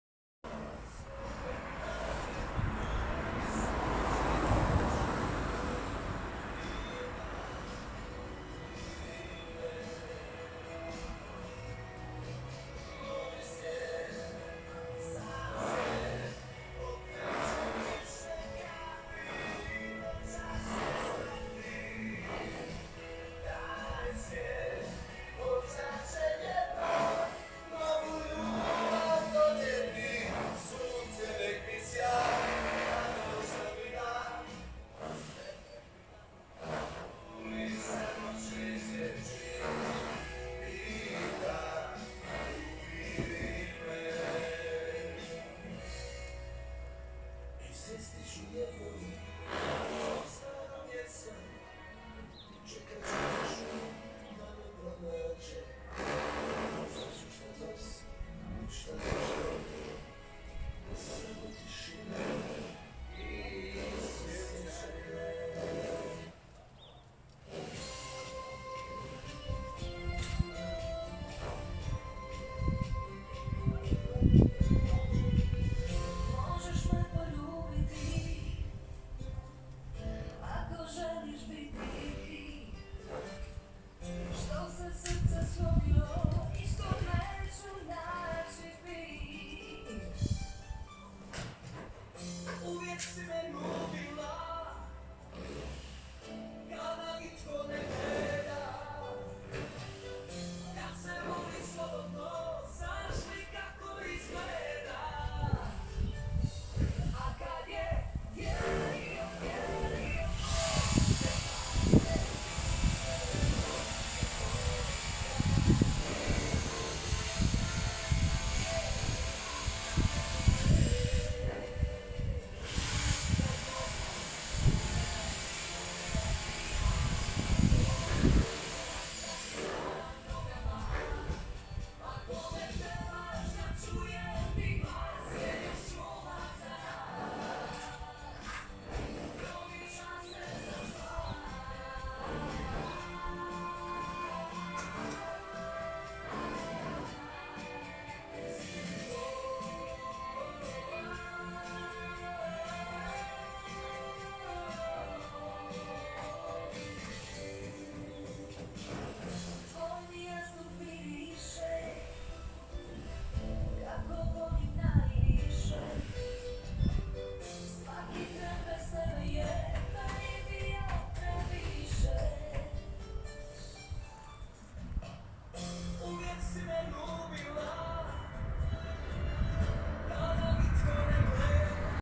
DJ SCREWDRIVER & WILD TURKS early morning performance 9/14/2022 at 9AM
Gift for neighbourhood due to adaptation of old house.

Drill villains early morning performance with DJ SCREWDRIVER & WILD TURKS 9/14/2022 at 9AM.